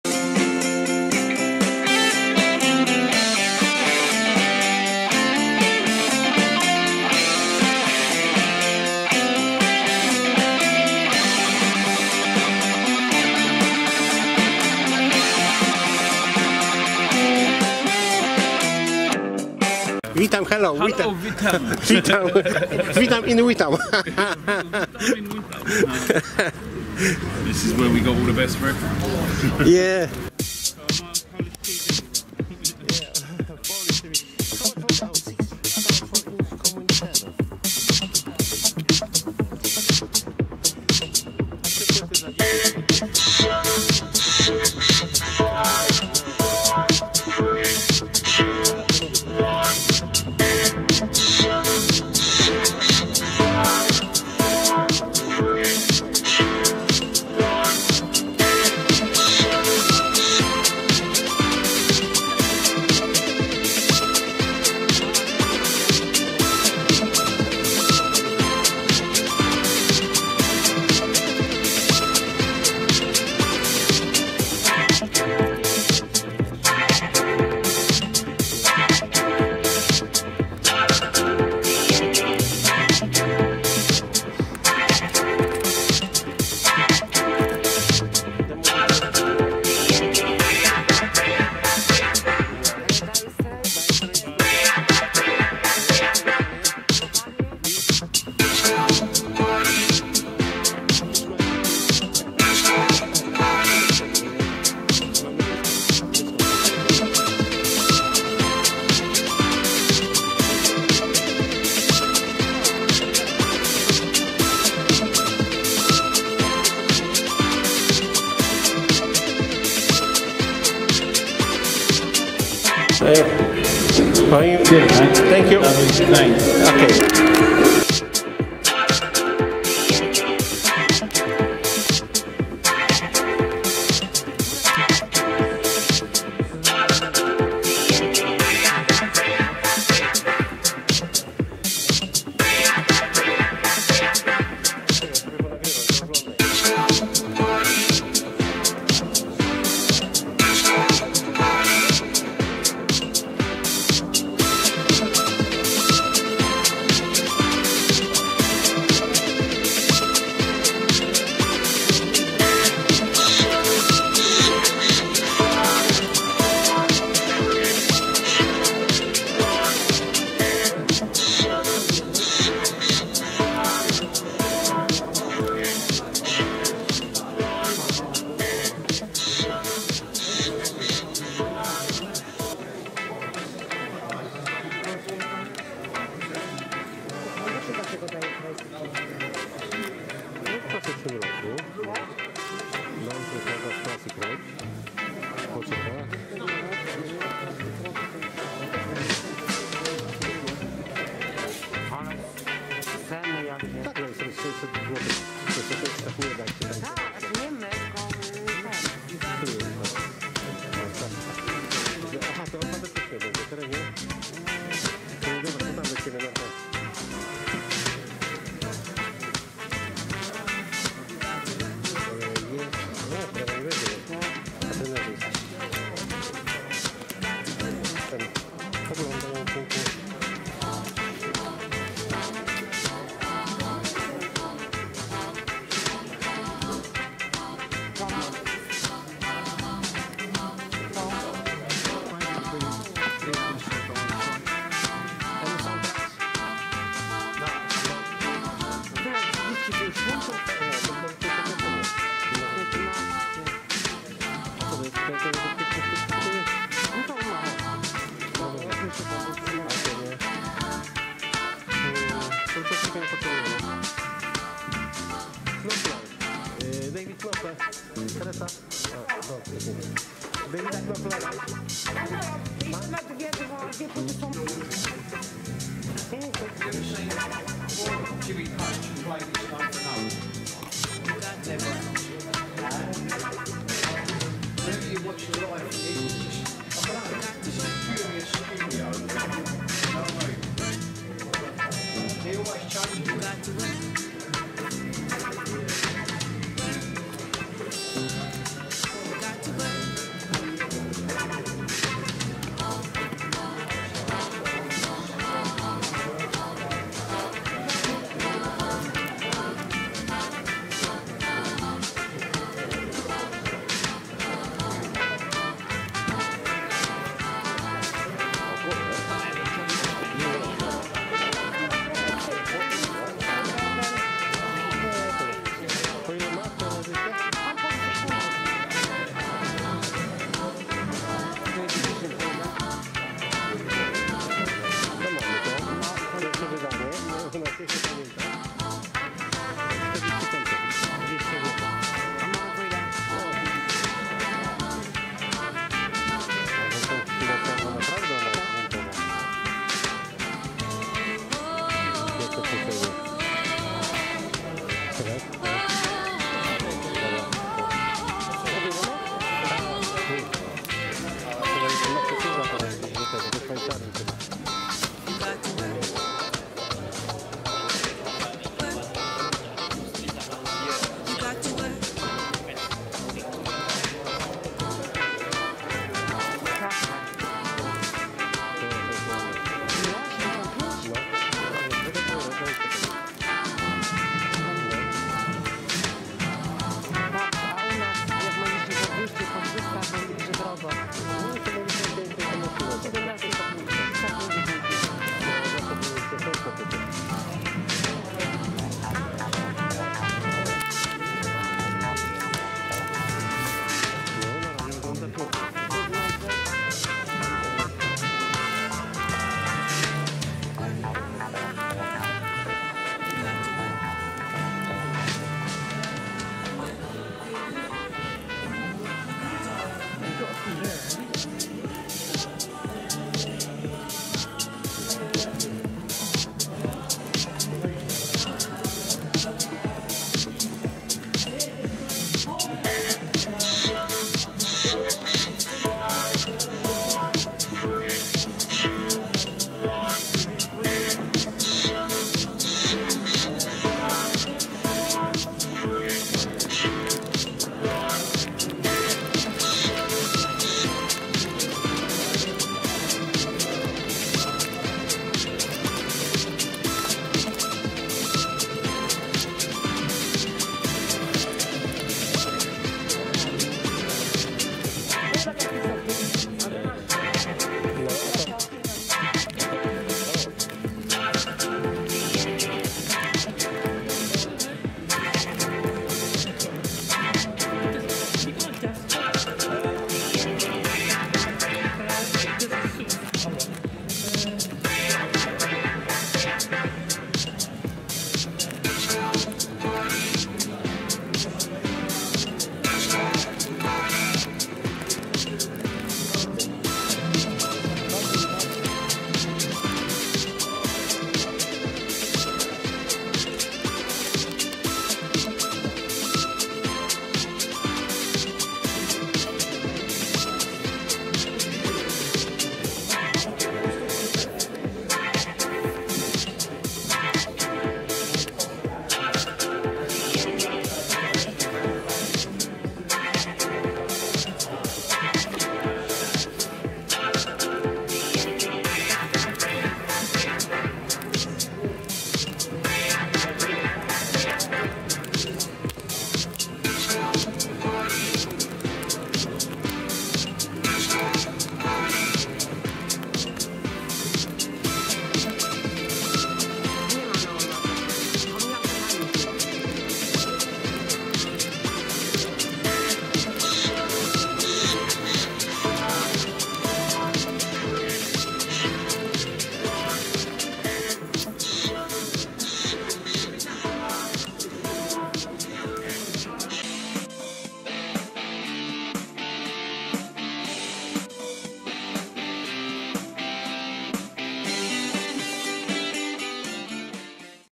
Giełda płyt winylowych w Witham
Witam na giełdzie płytowej w Whitam gdzie cena wjeściówki od 20-tu lat jest nie zmienna, 50p.od głowy.
Spedziliśmy tutaj około dwóch godzin w poszukiwaniu rodzynek. To co chcieliśmy nie kupiliśmy, tego czego nie było w naszych planach zakupowych kupiliśmy i zadowoleniy wyszliśmy.
Muzyka dzięki uprzejmości Pana Gucio.